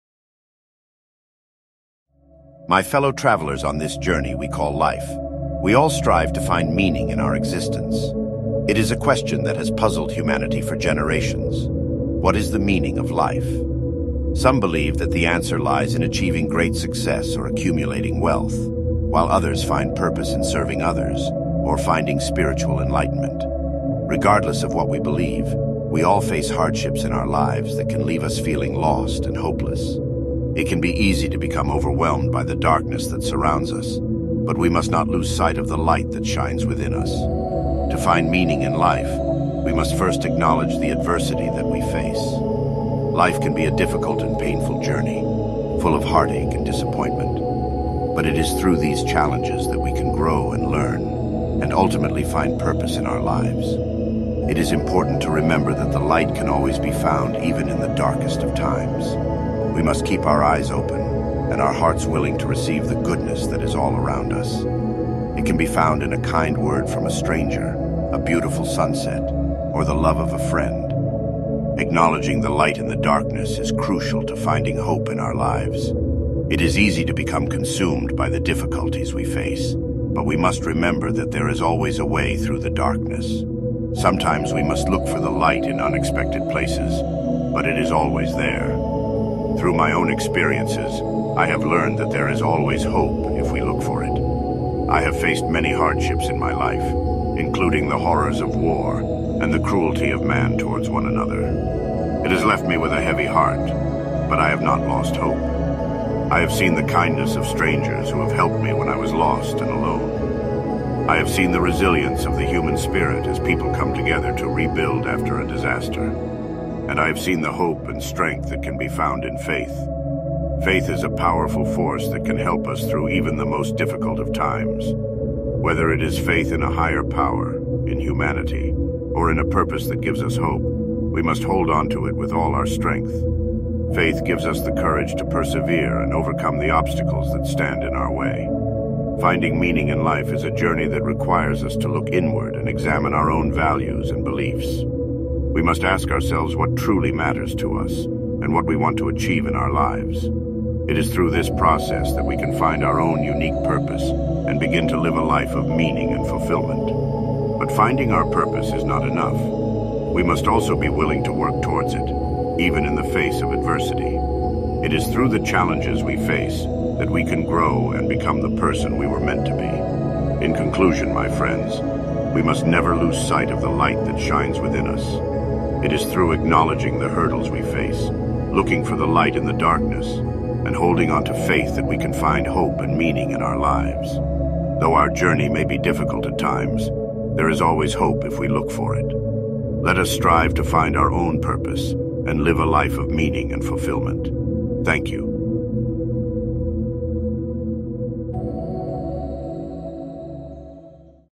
Joshua Graham AI: The Meaning of Life
Joshua Graham has always been a character that has fascinated me, primarily due to his unique philosophy. With the advent and growing competency of Voice AI technology, I thought it would be an interesting tool to use to illustrate and explore his character further.A huge thank you to the sources and inspirations listed below;

Script generation: ChatGPT
Thumbnail generation: DALL·E 2
AI voice generation: ElevenLabs